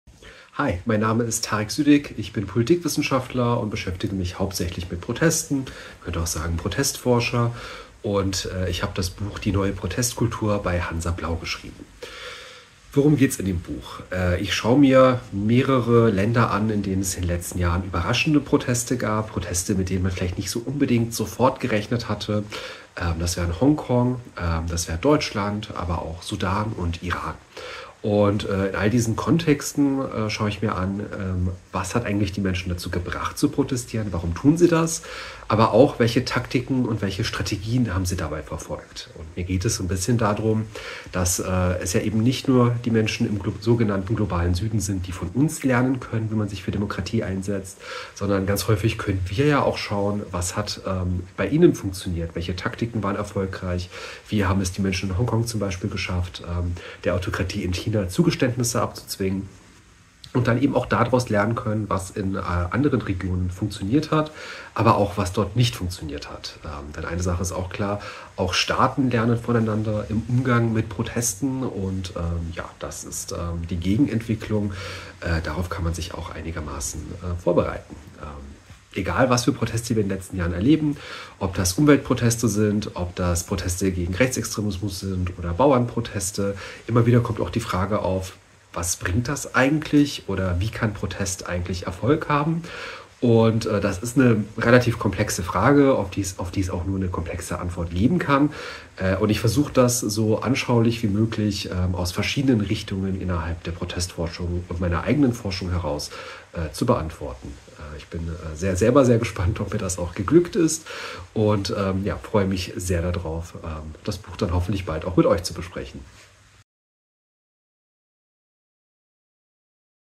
Tareq Sydiq über »Die neue Protestkultur. Besetzen, kleben, streiken: Der Kampf um die Zukunft«
Sie haben Regime gestürzt, Grenzen geöffnet und Rechte erkämpft – Proteste haben in der Geschichte schon einige Steine ins Rollen gebracht.
In der Gegenwart scheinen sie präsenter denn je, und es hat sich verändert, wer wofür oder wogegen und in welcher Form protestiert: von Frauen, die ihre Kopftücher verbrennen, über Bauern, die mit ihren Traktoren Straßen blockieren, oder Klimaaktivist:innen, die sich am Boden festkleben, bis hin zu Social-Media-Posts unter Hashtags wie GegenRechts, MeToo oder MutZurWahrheit.

Der Protestforscher Tareq Sydiq beleuchtet in seinem Buch die neue Protestkultur anhand zahlreicher Beispiele. Anschaulich und augenöffnend zeigt er, was diese Proteste ausmachen und wie sie die Politik und Gesellschaft beeinflussen.